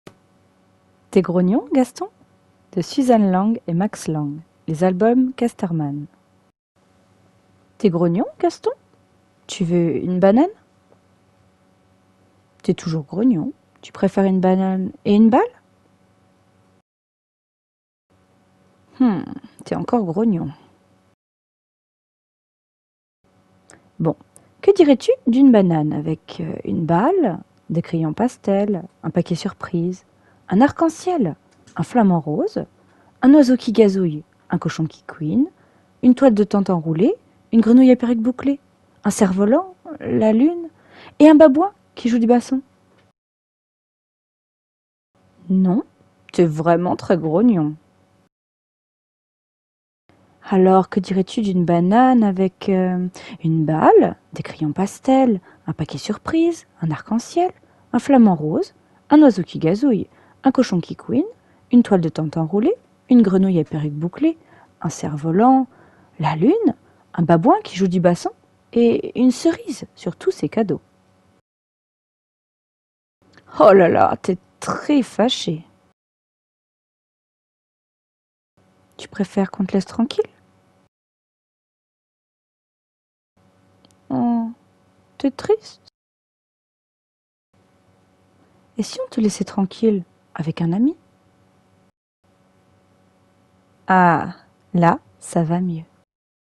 Ma Lecture... T'es Grognon, Gaston ? 🐒
T'ES GROGNON, GASTON ?
Un livre de Suzanne Lang et Max Lang.
Les Albums Casteman.

Vidéo enregistrée sur Sharefactory.

Merci de faire prendre vie a mon projet ❤️